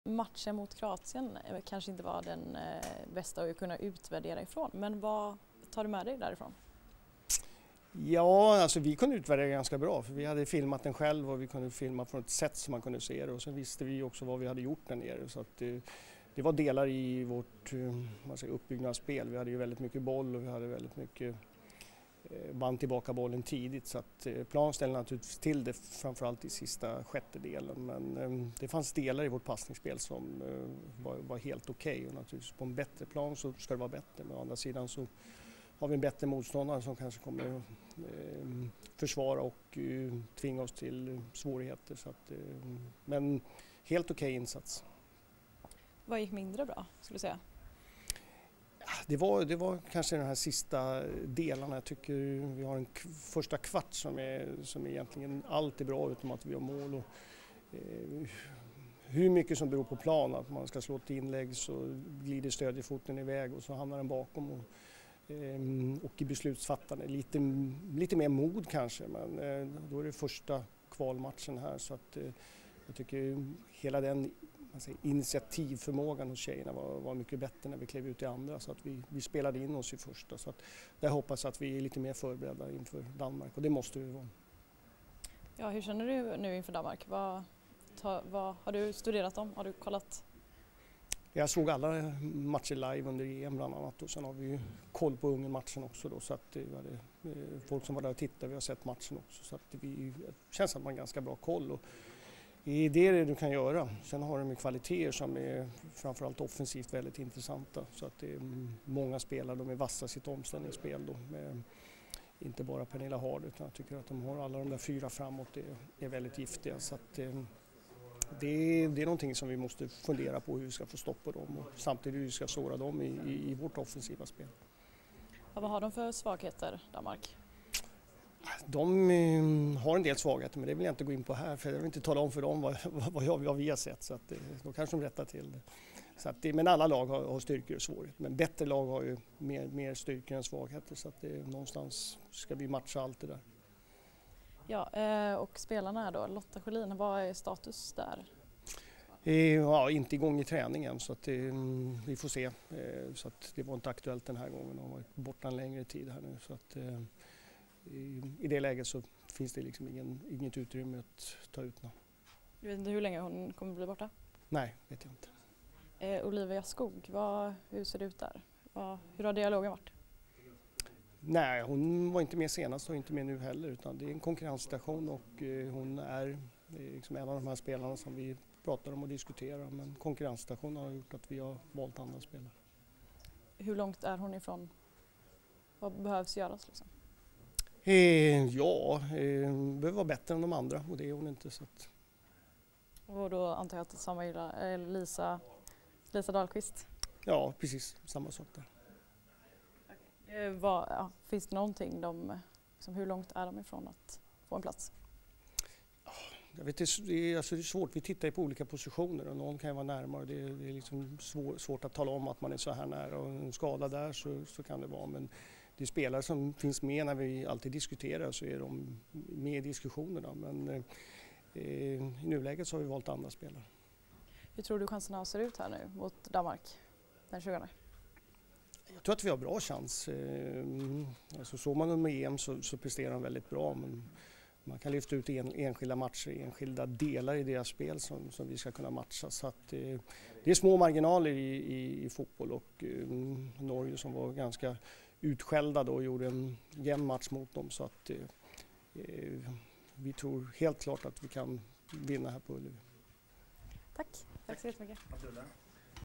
Gerhardsson inför mötet mot Danmark: "Vi tror helt klart att vi kan vinna" - TV4 Sport
TV4 Sport från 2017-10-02: Förbundskaptenen Peter Gerhardsson om matchen mot Kroarien, petade spelare och inför mötet mot Danmark.